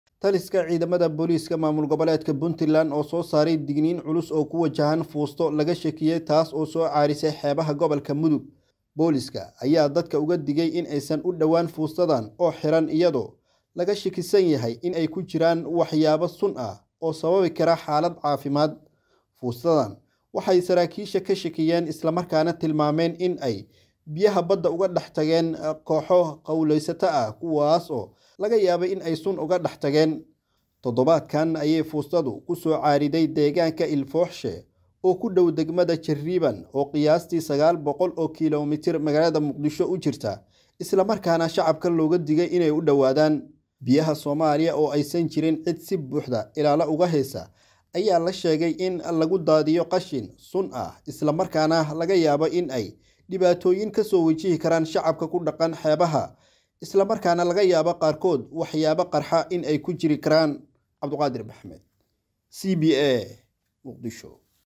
Booliska maamulka Puntland ayaa digniin culus kasoo saaray foosto laga shakiyay oo kusoo caariday...
Booliska maamulka Puntland ayaa digniin culus kasoo saaray foosto laga shakiyay oo kusoo caariday xeebaha gobolka Mudug.